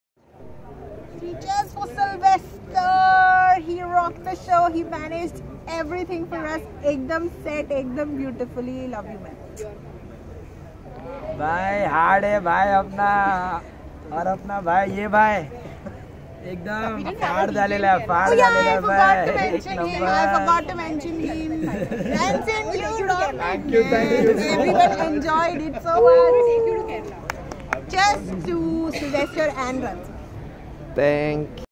Post wedding tales Subin & Sheril
Post wedding tales
When the couple appreciates the Compere and the Dj as well.

Subin & Sheril

like4follow - Anchor/ Emcee/ Compere Pontes